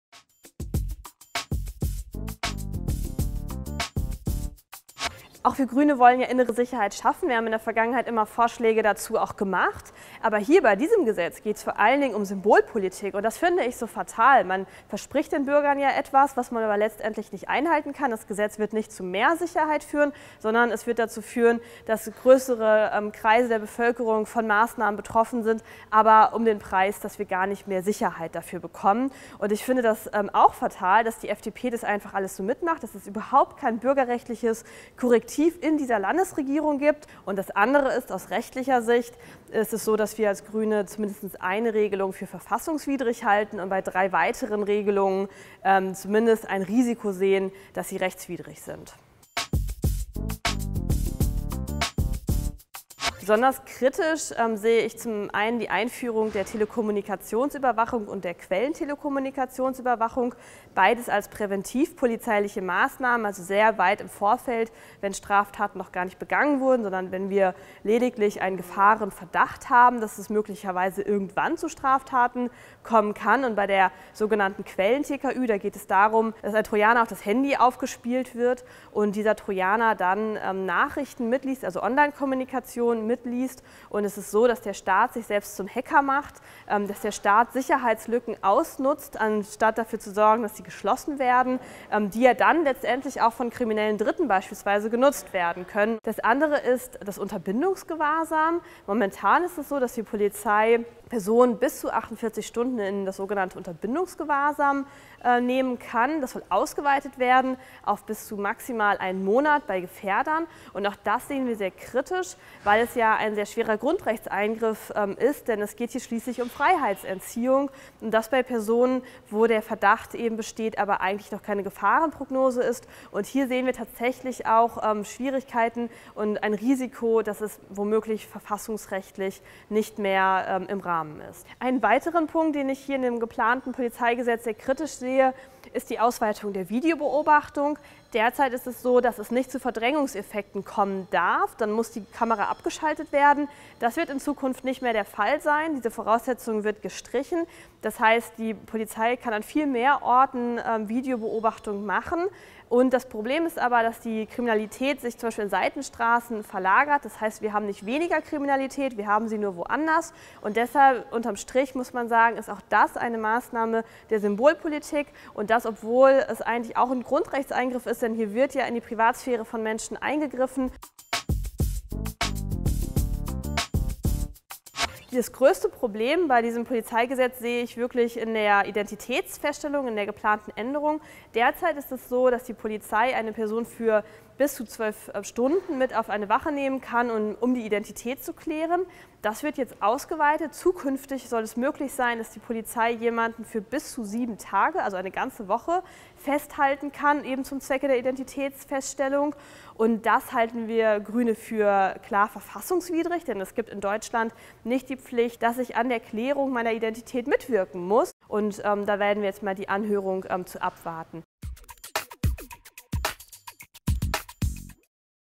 Verena Schäffer zu den geplanten Änderungen am NRW-Polizeigesetz
Viel Symbolpolitik und weniger Rechtsstaat: Verena Schäffer hat heute die Bewertung der schwarz-gelben Änderungen des Polizeigesetzes vorgestellt, das die Landesregierung nächste Woche in den Landtag einbringt.